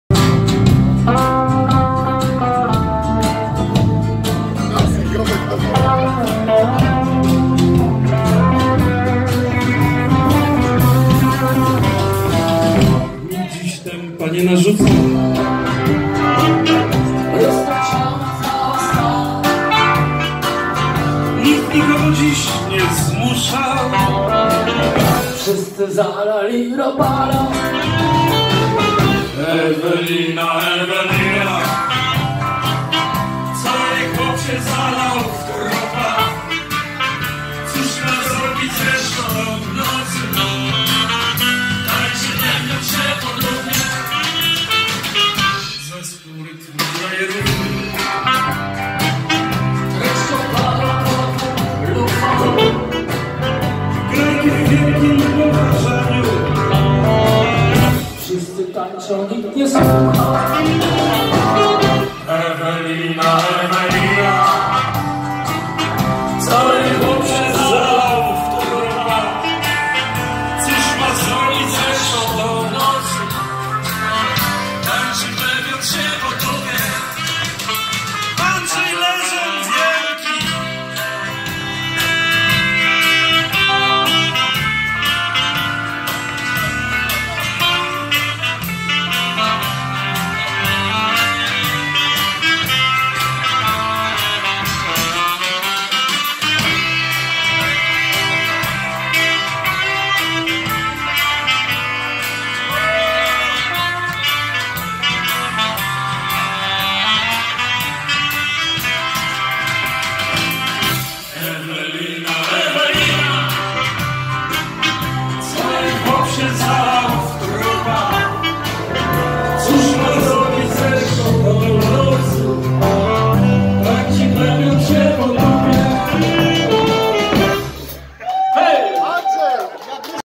Ewelina - Bardziej
Ewelina i kapela "Bardziej" Grzesiek Grzegorz Dąbrowski i Paweł Krokuś wokal, Tobiasz Wawrzyniak - bębny, Andrzej Leżoń saksofon, Michał Jezierski - bas, gościnnie Paweł Kanonowicz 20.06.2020 Poprawiny w klubie Bajka w Obornikach Śląskich